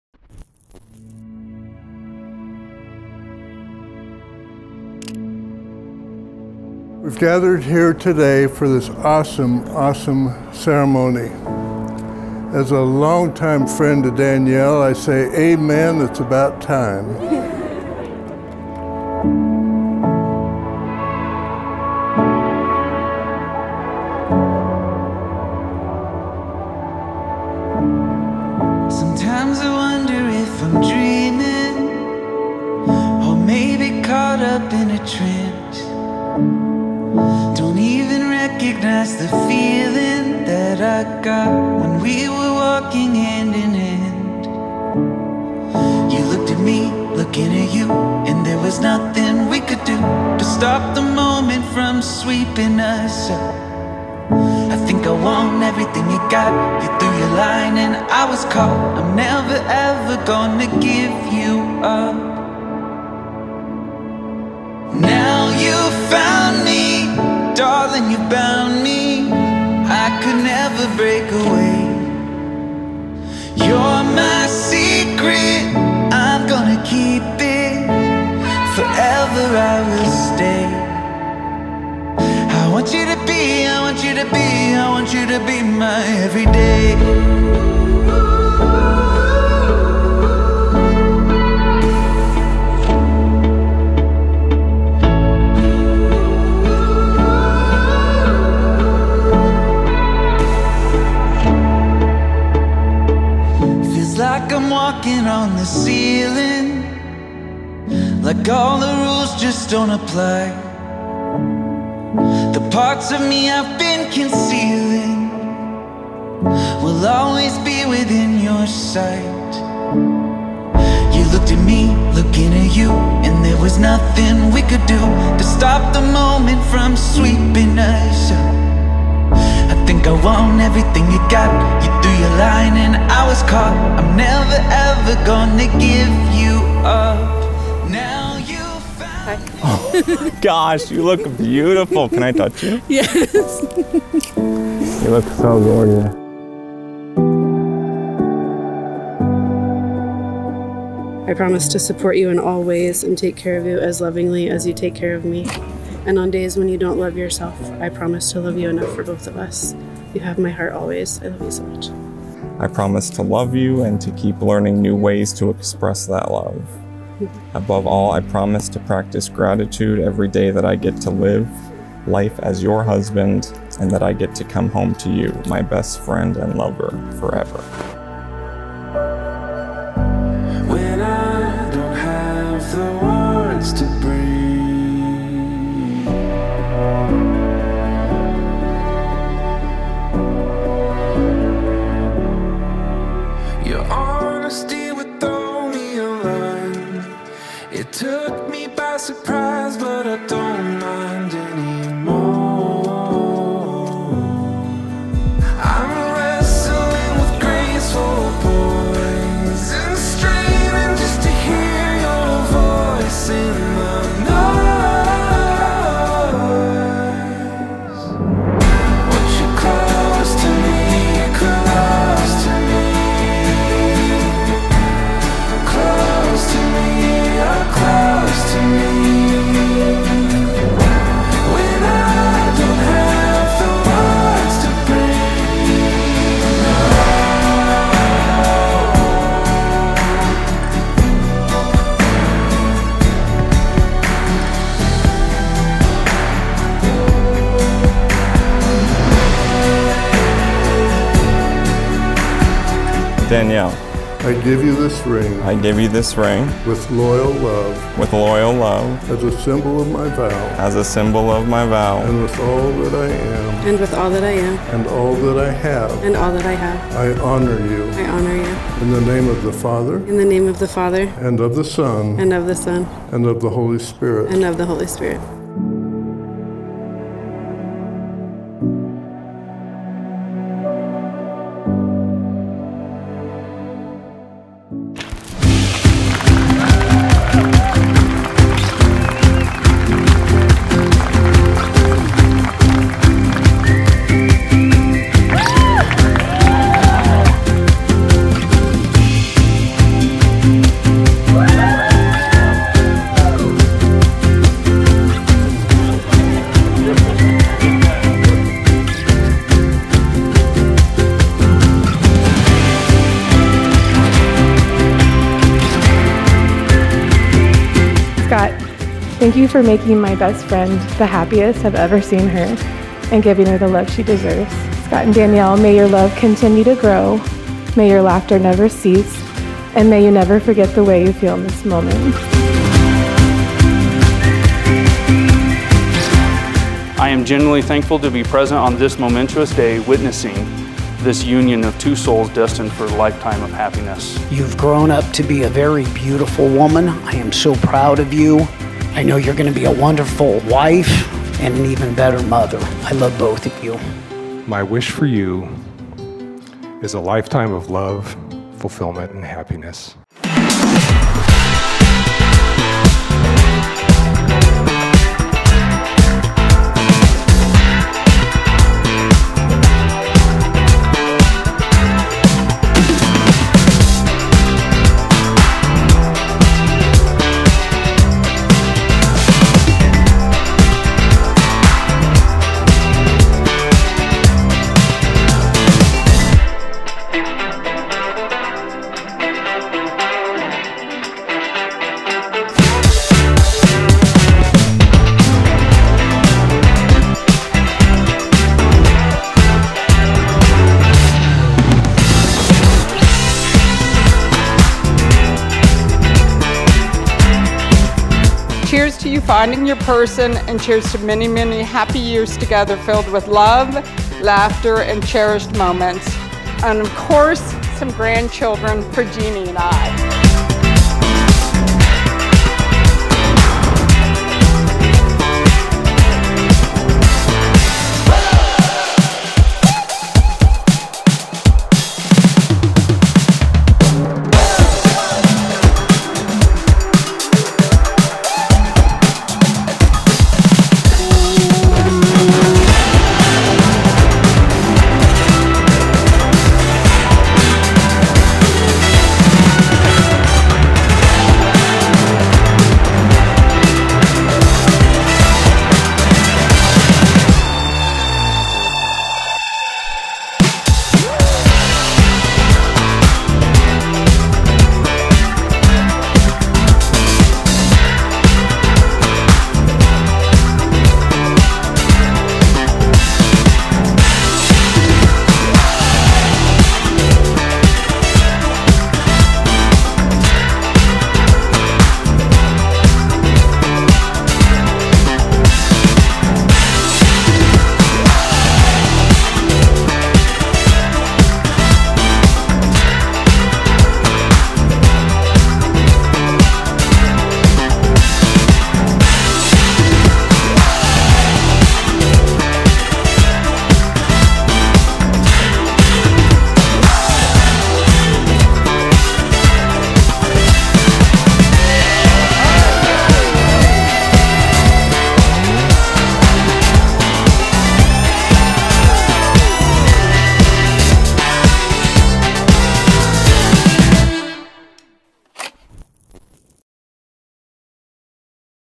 Scott + Danielle | Cinematic Wedding Film
The wedding of Scott & Danielle.

Gather Estate, Mesa, AZ. November 24th, 2023

Shot on Canon R6 Mark II & Panasonic Lumix S1